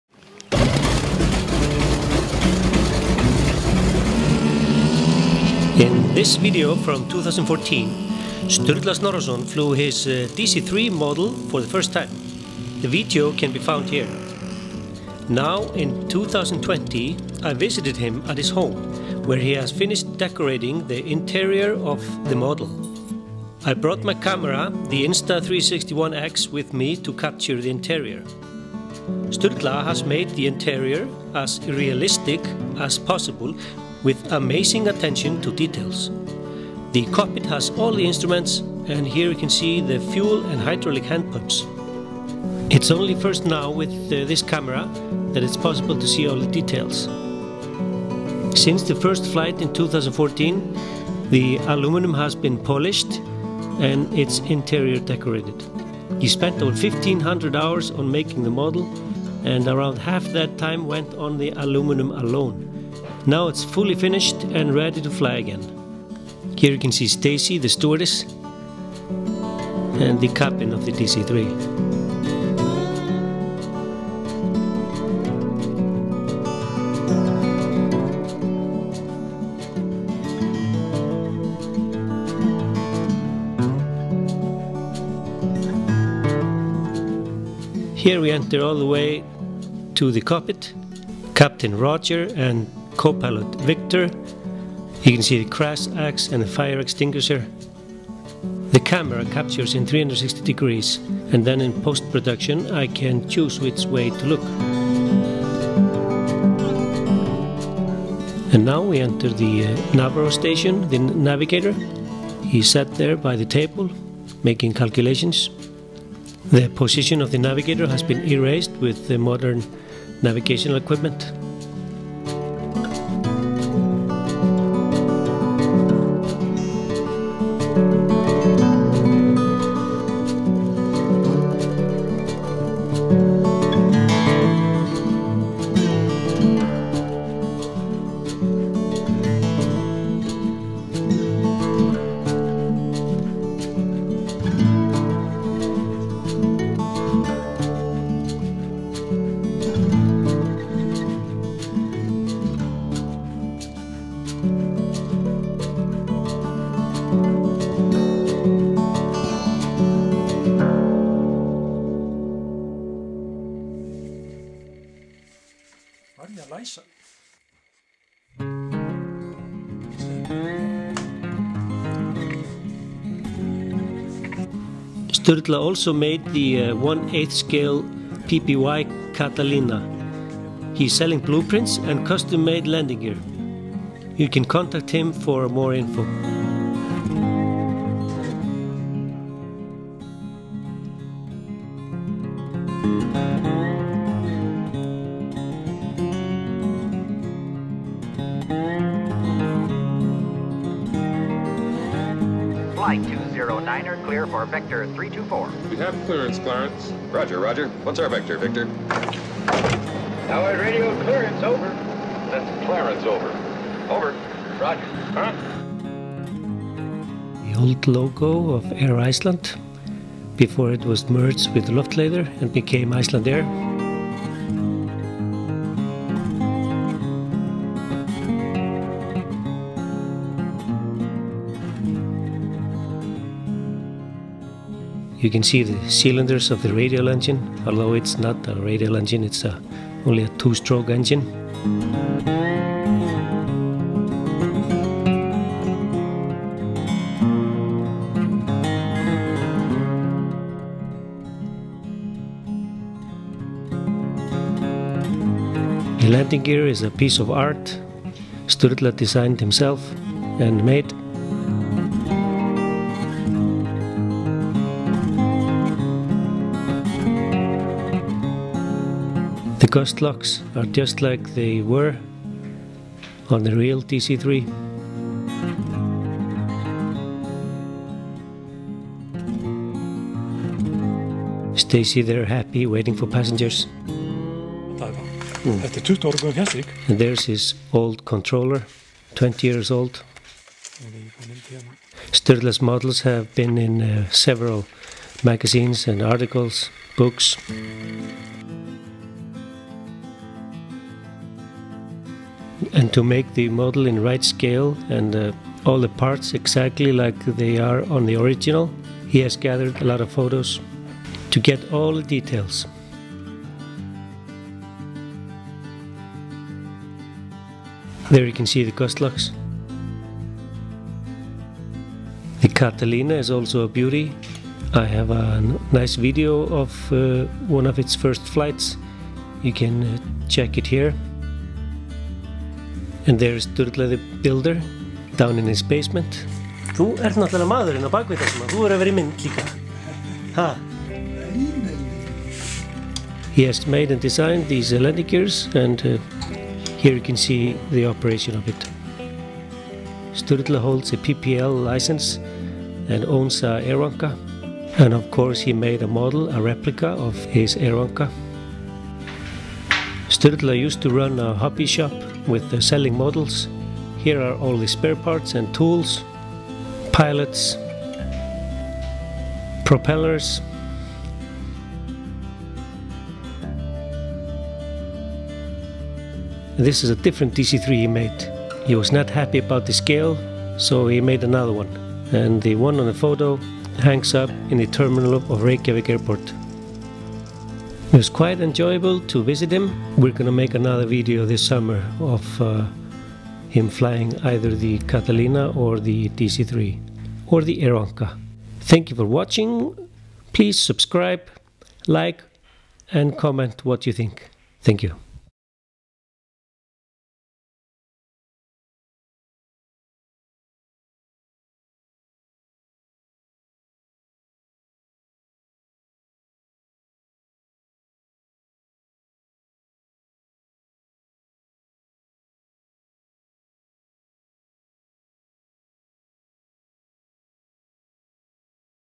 Giant DC-3 RC Interior!
This is a beautiful RC model of DC-3 / C-47 in. Flugfélag Íslands (Air Iceland) operated DC-3´s from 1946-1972 in this color scheme.
Here is Sturla´s description:
"This model is more than a simple piece of shiny real aluminium work. It's in perfect 1/8 scale, nose, engine house, tail, length and wings are all in 1/8 scale. It has the right airflow for all the steerings so it has realistic flight. It was made by my own design, as all other designs I found out there were out of scale on some parts, and as a big enthusiast of this plane that wasn't simply good enough for me. It was an incredible experience to see it take it's first flight back in 2014, it took total of fifteen flights.  Since then the aluminium has been polished and it's interior decorated. I've spent over 1500 hours on making the model and around half that time went on the aluminium alone. Now it's fully finished and ready to fly again".
He has also made a 1/8 scale PBY Catalina.  He is selling blueprints and custom made landing gear.  Avião de controle remoto gigante